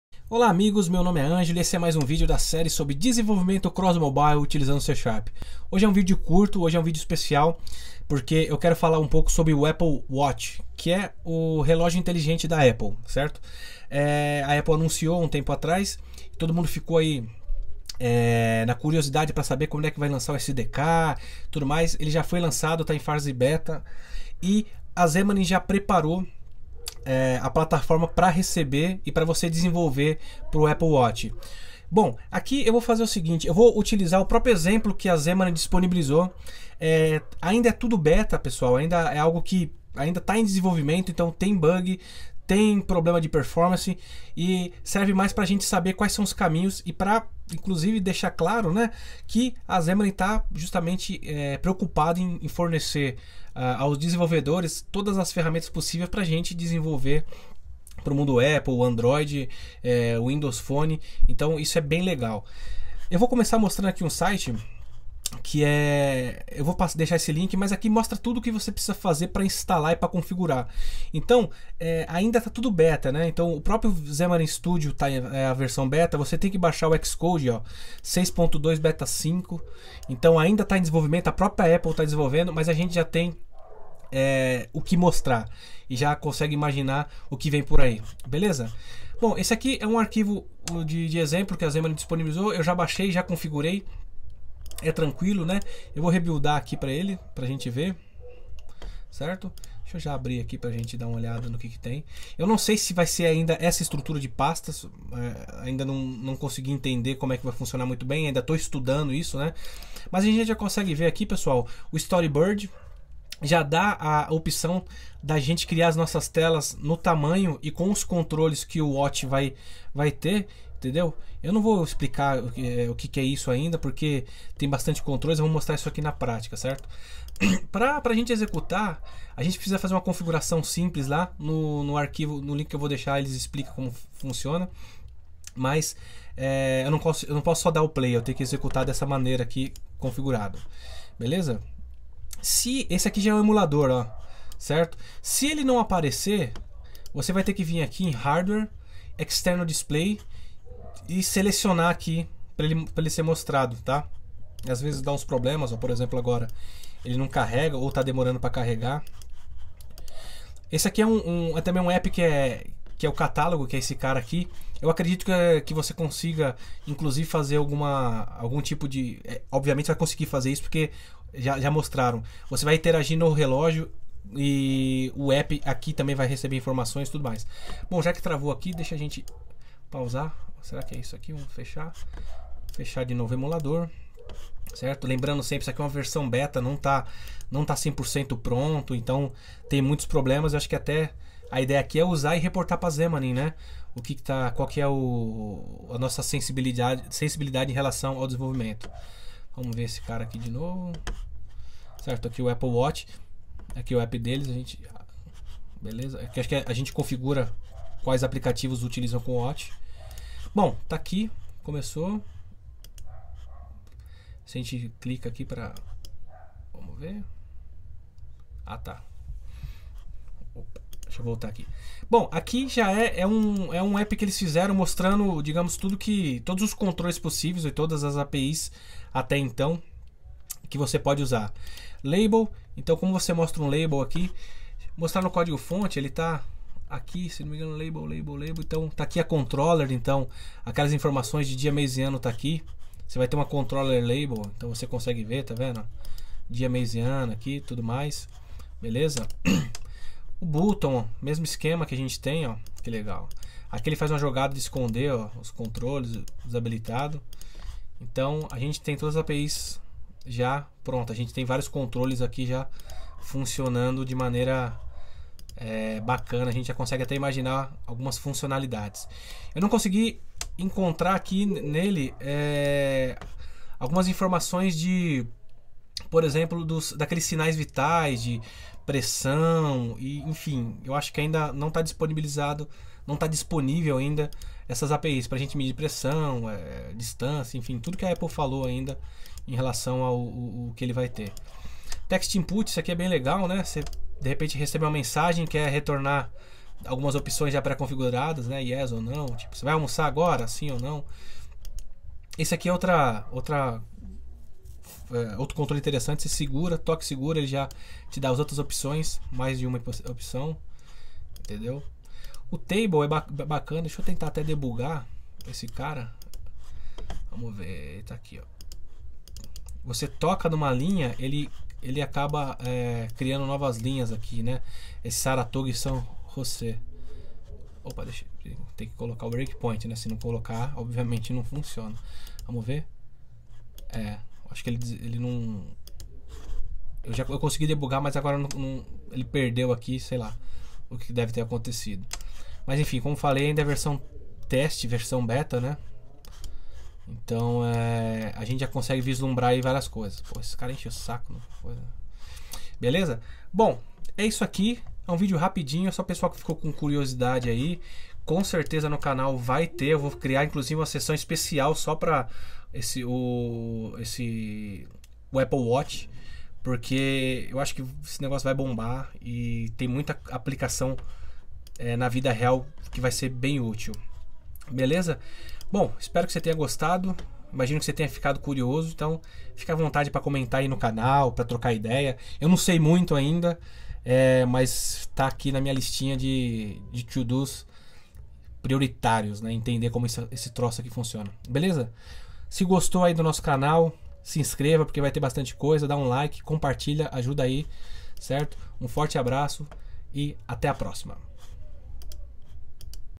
Apple Watch e Xamarin: O que vem por ai!
Já é possível criar seu app para o Apple Watch utilizando C# com o Xamarin. Nesse curto vídeo eu mostro um exemplo.
Esse projeto foi feito pela equipe da Xamarin para testes!